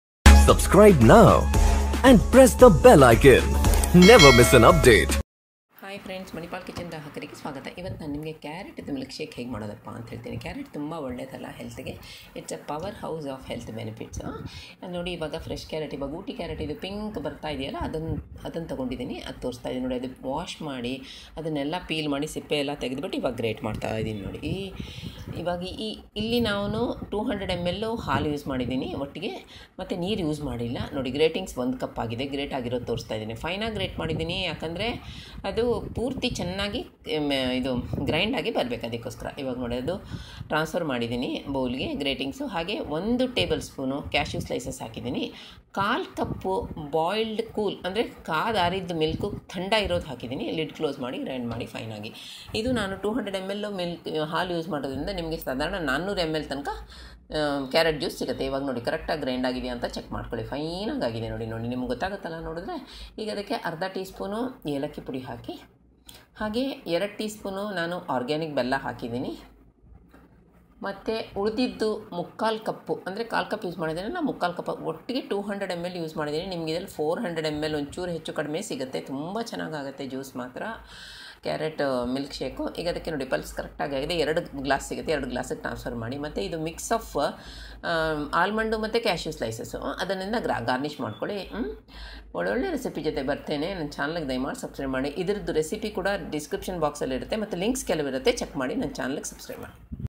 Carrot Milkshake |ಆರೋಗ್ಯಕರವಾದ ಕ್ಯಾರೆಟ್ ಮಿಲ್ಕ್ ಶೇಕ್| Manipal Kitchen
Carrot is a good source of beta carotene and   fiber, vitamin K , potassium and antioxidants.  They have a number of health benefits and they are weight loss friendly food and have been linked to lower cholesterol levels and improved eye health.  It is widely used in most of the pulav recipes and also in salads and milkshake not only for adults but also for kids.  Here goes the recipe for carrot milkshake.
 INGREDIENTS :
1. Grated carrots - 1 cups
2. Boiled cool milk - 1 cup (200 ml)
3. Cashews slices -1 tbsp
4. Cardamom powder - 1/2 tsp
5. Organic jaggery - 2 tsp
6. Cashew & almond gratings - 2 tsp (to garnish)
PROCEDURE: Wash, peel and grate carrot fine with the help of a small gratings plate.  Transfer to the blender bowl.  Add cashew slices and also 1/4 cup milk and pulse fine as there is no straining. Check and add cardamom powder, organic jaggery and remaining milk and pulse very fine.  Transfer to serving glasses and garnish with cashew and almond slices and enjoy.  Yum Yum!
Yields - 400ml (2 glasses)
Other Related Links On This Channel:
1. (Musk mellon milk shake)
(Oats smoothie , no sugar , no diary, a weight loss recipe)
2. ( Apple-oats & dates smoothie a weight loss recipe)